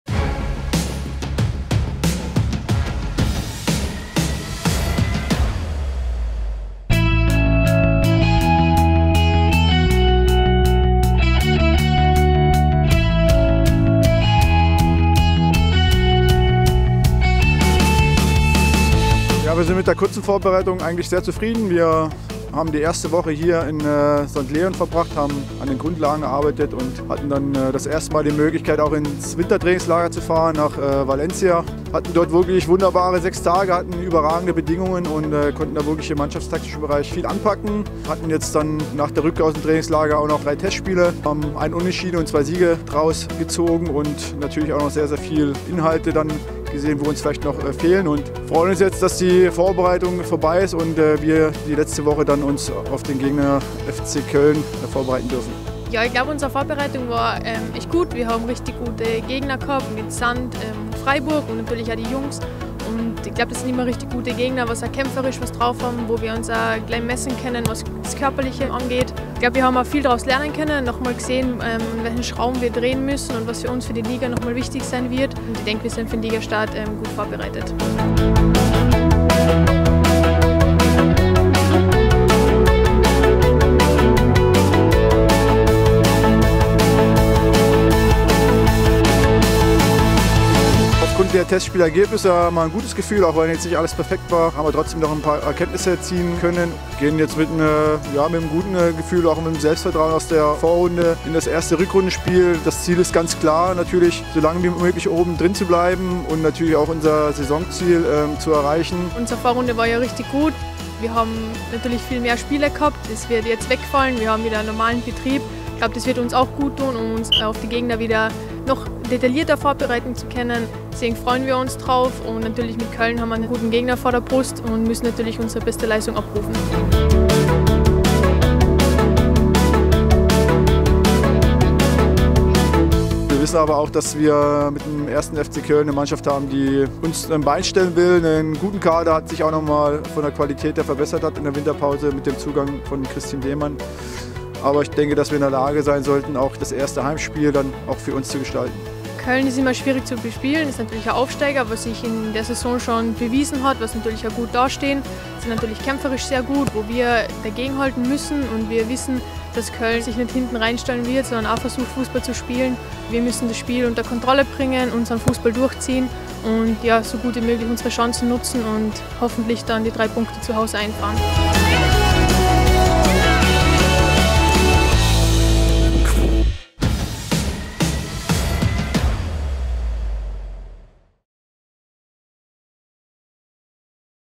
"Wir sind mit der Vorbereitung sehr zufrieden" | Rückrunden-Start TSG Frauen
Am kommenden Freitag (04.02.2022) empfangen unsere TSG Frauen den 1.FC Köln zum ersten Pflichtspiel des Jahres. Anpfiff im Dietmar-Hopp-Stadion ist um 19:15 Uhr. Wie die Vorbereitung lief, verrieten uns Abwehrspielerin Katharina Naschenweng und Cheftrainer Gabor Gallai.